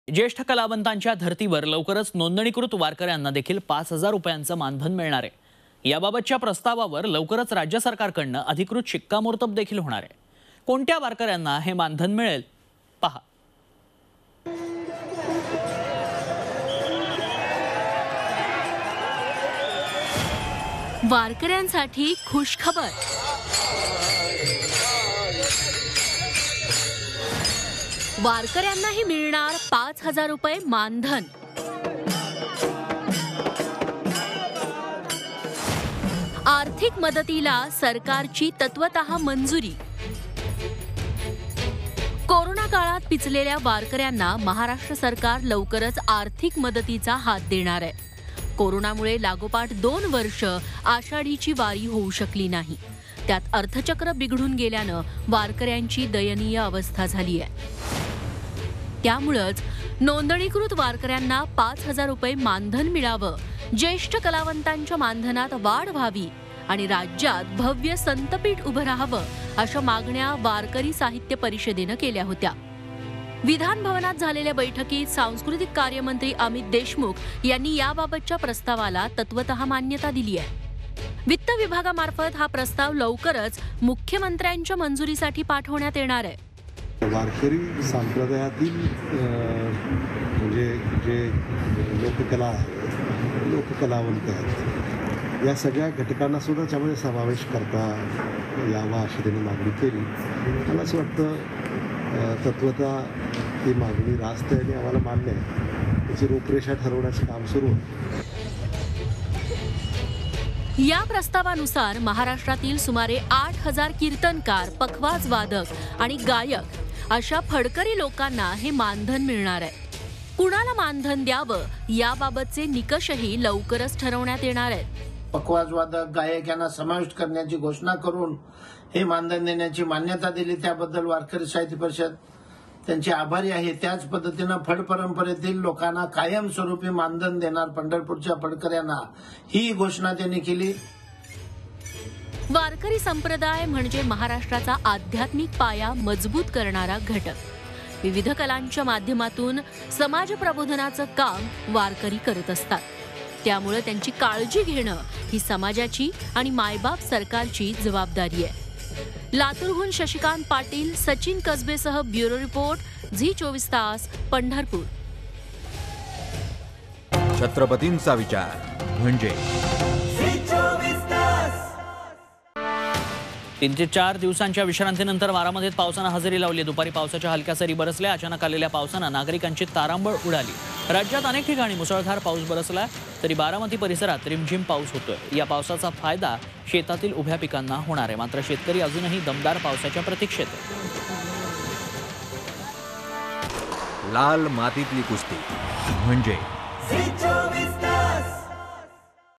Special report | वारक-यांसाठी खूशखबर ! मिळणार 5 हजार रुपये मानधन
Report On Warkari will get 5000 rs Honorarium